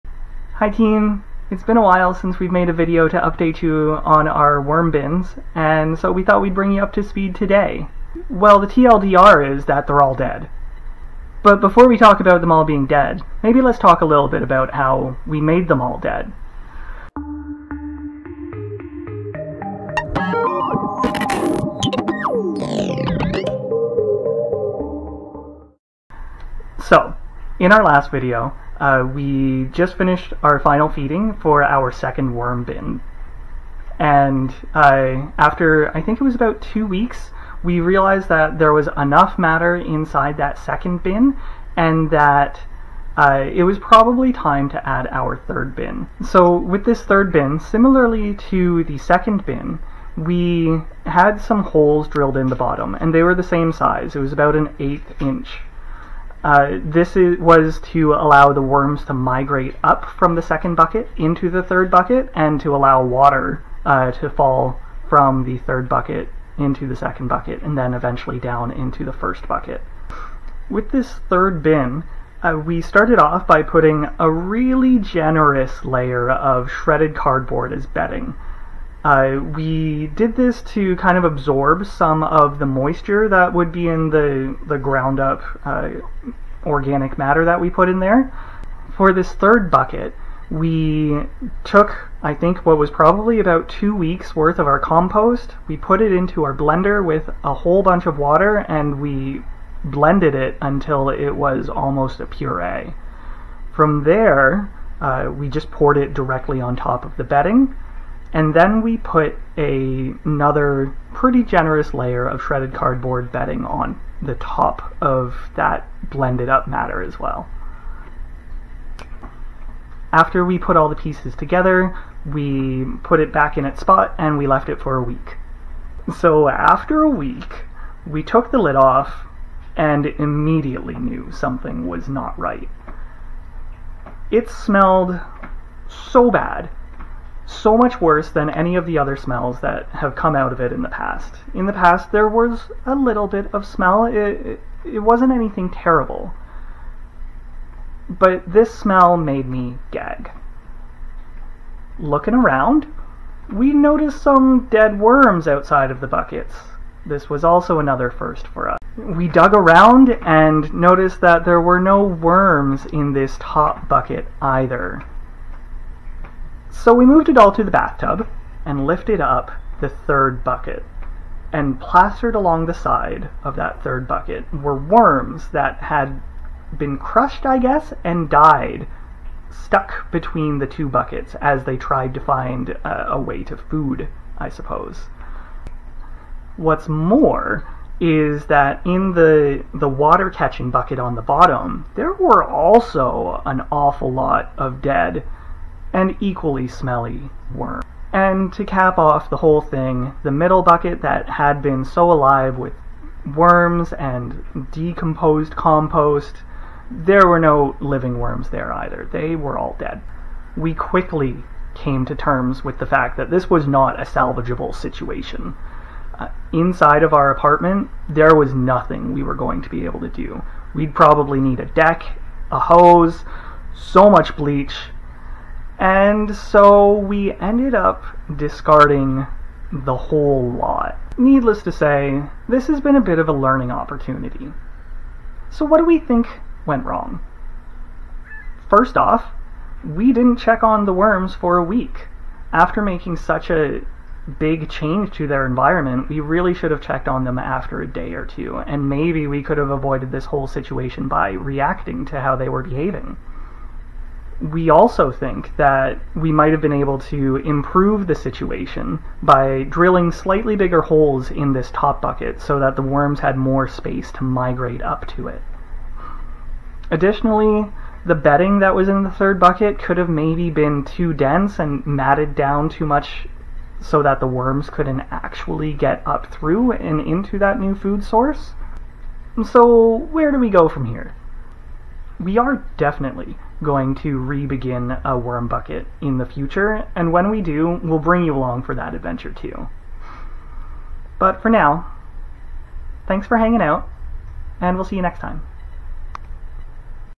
Not the vermiculture update we wanted to give: We've lost all our worms | What went wrong?
*** Major bummer alert, but the video does not contain images of the failed apartment vermiculture compost nor any of the dead worms from our once thriving worm bin. ***

This all happened a couple of weeks ago but with the panini, life, work, etc. and general sadness it took us a long time to put this video together.

What happened? As far as we can tell the worms failed to move from the established second layer bucket into the new top layer bucket. We aren't sure if it was the size or position of the holes, or if the bedding between the two layers prevented their movement.

One major lesson we learned is that is we make a change to their environment, we need to check in on them more frequently. We should not have let a week go by between adding the third bucket and observing their progress.

We have decided to take a break before starting up a new bin. Our apartment now has a green bin (organics disposal) so the urgency to compost is a bit less. We also want to make sure we get it right the next time and don't harm our worm friends. We aren't quite sure what the next bin will look like, but it will probably be closer to a larger bin system, maybe with chicken wire or a larger mesh separating the layers so that there is better air flow and worm mobility.

Another big reason we don't want to start again is that we will be moving in 2 months (!) and don't want to have to move the bin with us. Our new place will be slightly larger and also give us the opportunity to have an outdoor compost pile 👀